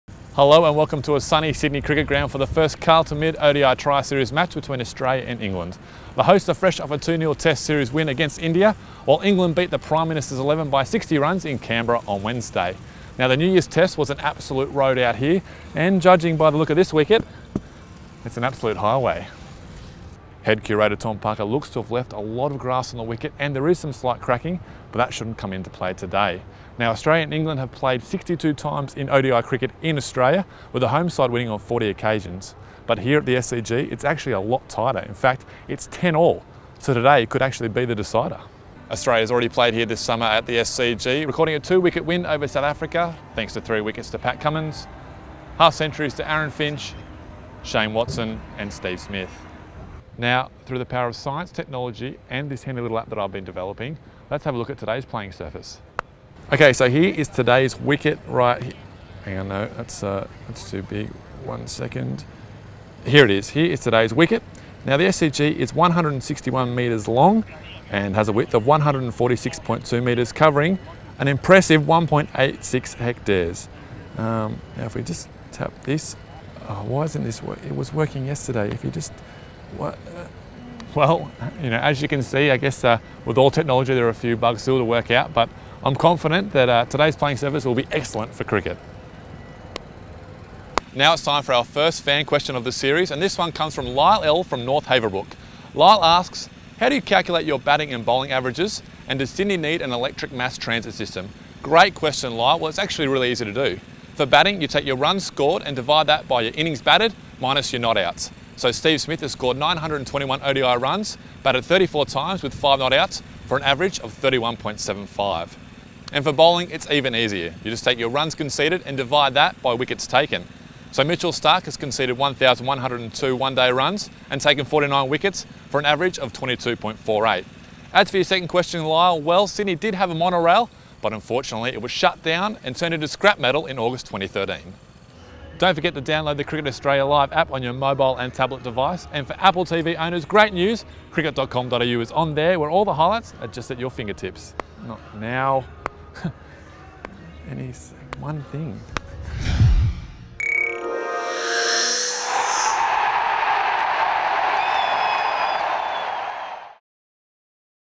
Runs expected on SCG highway
Sam Ferris lets fans in on everything they need to know ahead of the first match of the Carlton Mid ODI Tri-Series at the SCG